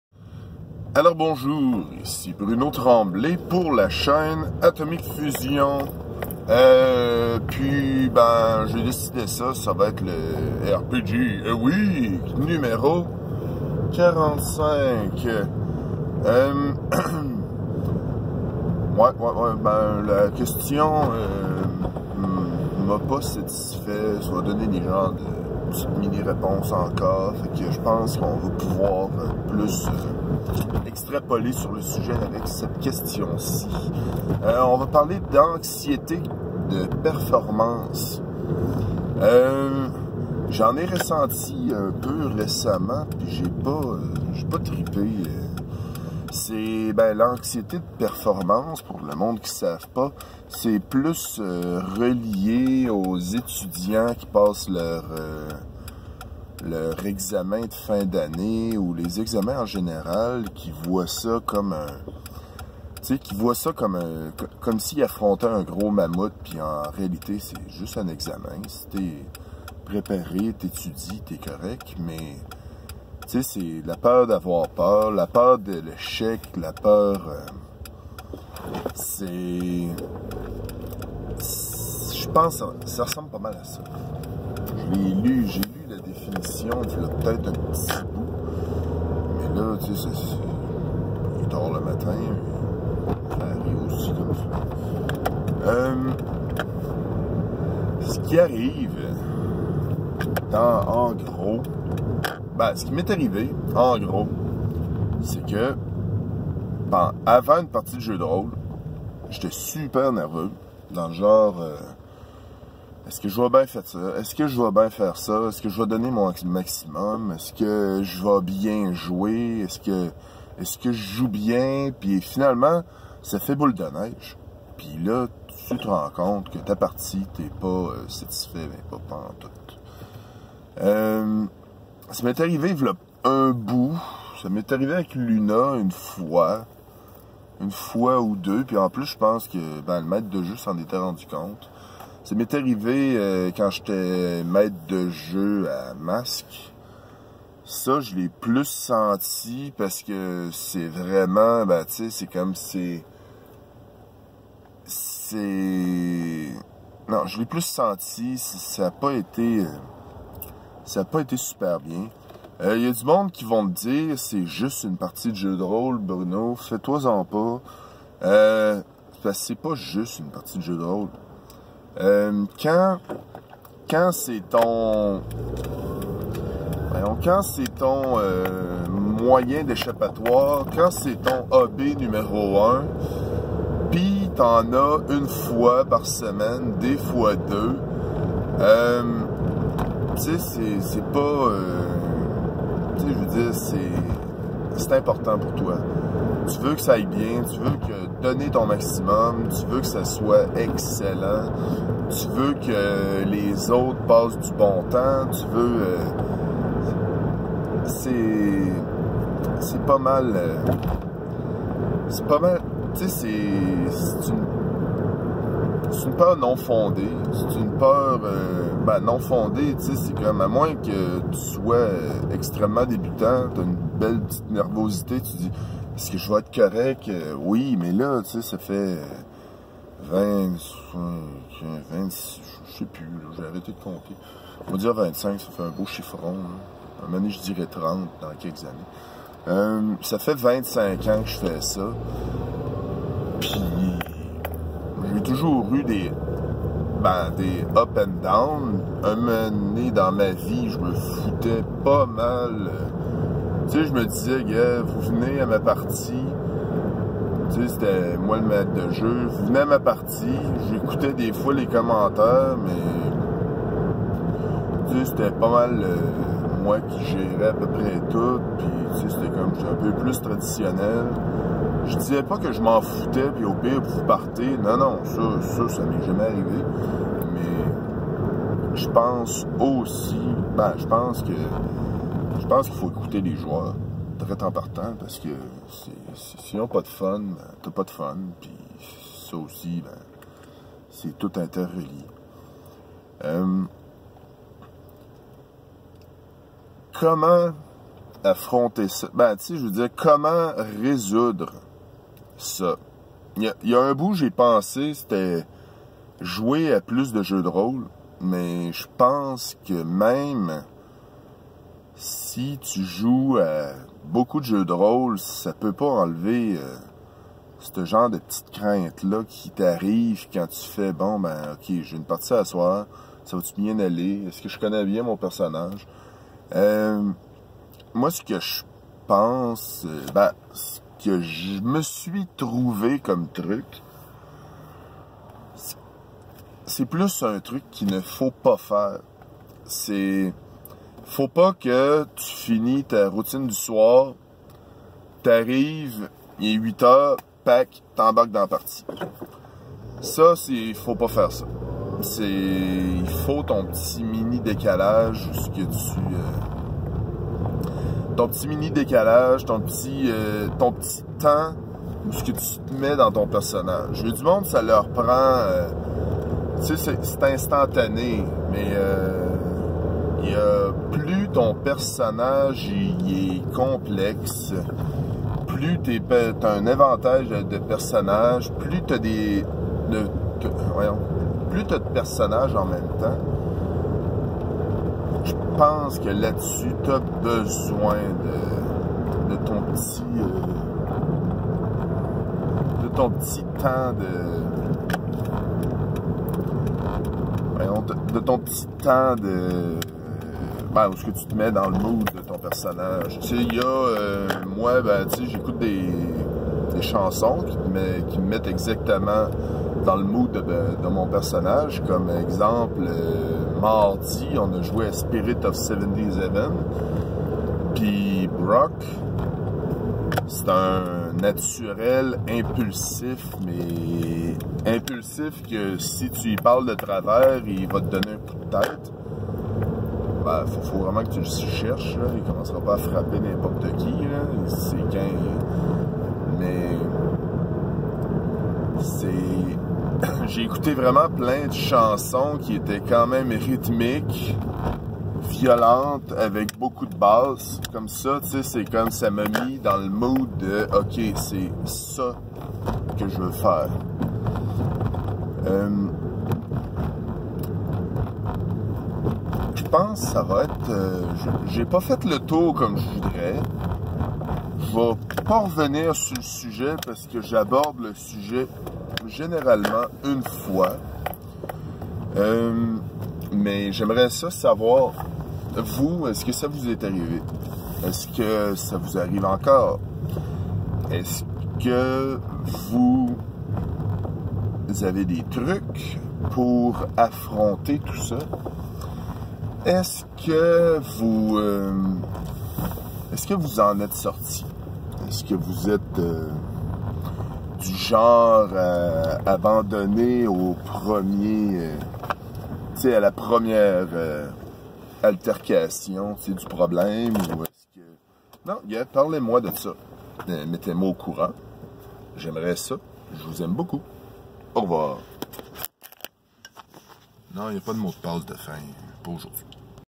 l'anxiété de performance
Vous est-il déjà arrivé de ressentir de l'anxiété de performance?
Avoir peur de ne pas livrer la marchandise en jdr?
En tant que joueur ou maître de jeu?
Commentez.